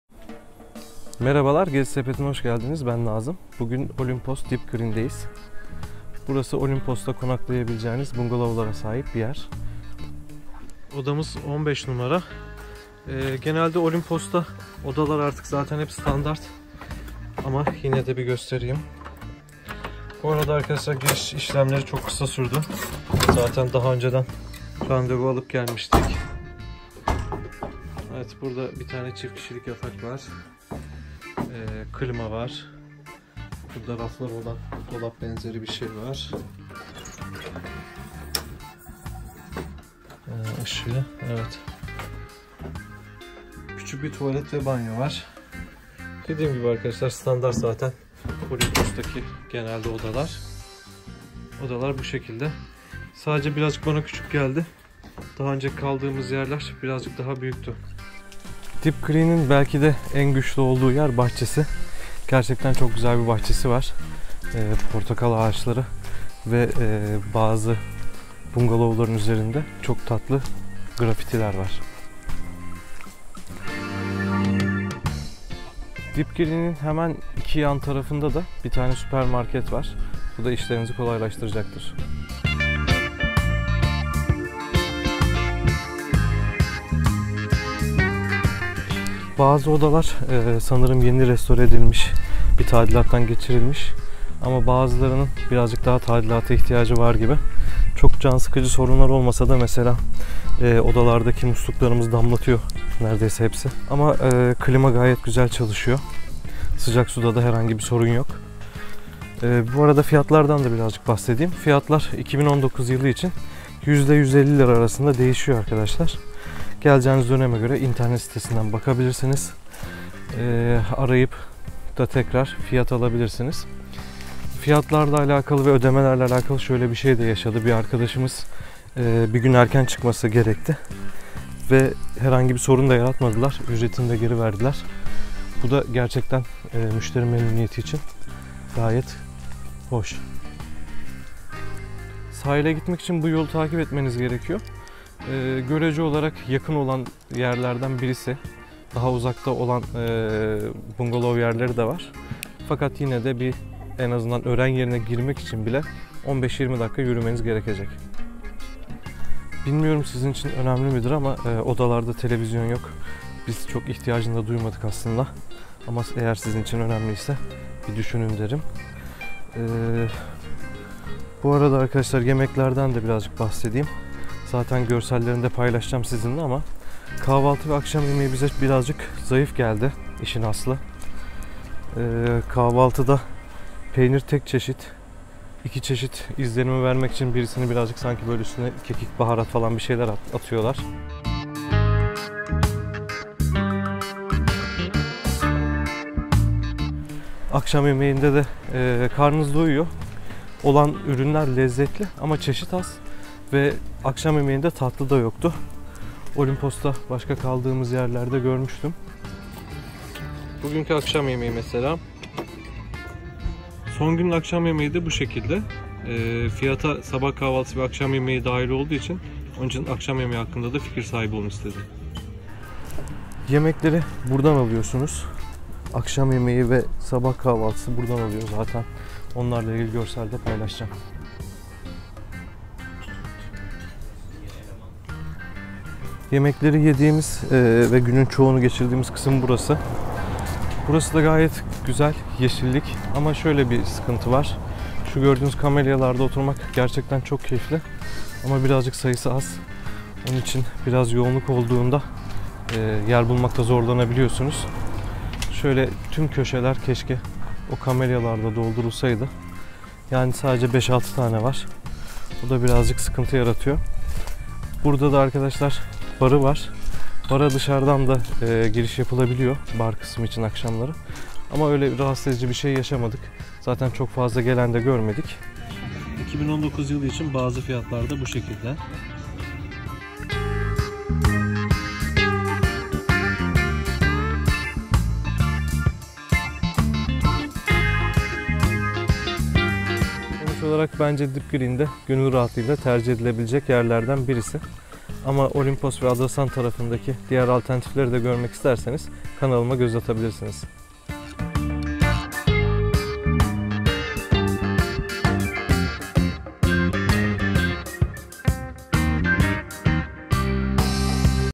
Olimpos(Olympos) Deep Green Bungalows Tanıtım | Olympos Pansiyon Öneri
Olympos'da konaklama önerisi. Pansiyon-Bungalow . Olympos'da nerede kalınır. Pansiyon fiyatları. Bungalow fiyatları. Olimpos ağaç evleri. Konaklamak için tercih ettiğimiz Deep  Green Bungalow'u sizler için tanıttım. Umarım beğenirsiniz.